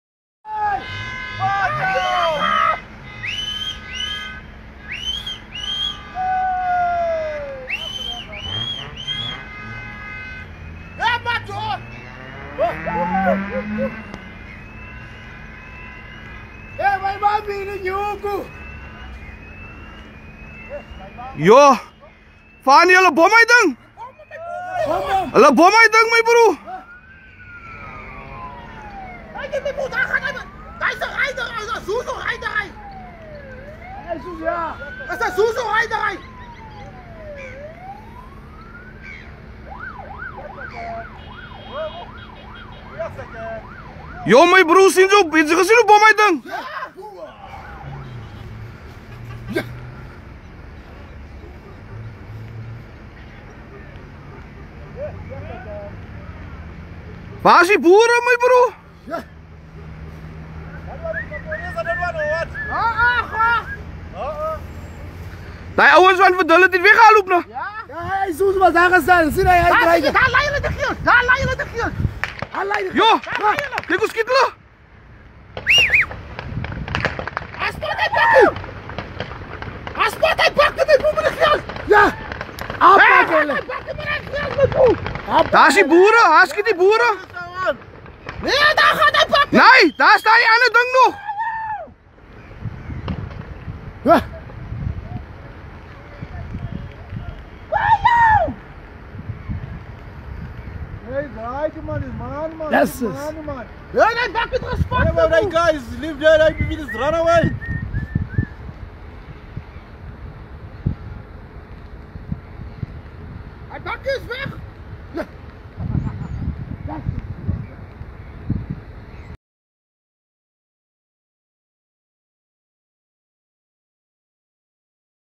Cape Town cash-in-transit heist captured on camera
Another cash van was ambushed on June 18, 2018, this time in Cape Town during the morning rush hour. One of the hundreds of bystanders captured the attack on video. Police spokesperson Lieutenant Colonel Brenda Muridili confirmed that a cash-in-transit incident had taken place on Jakes Gerwel Drive but could not yet provide further details.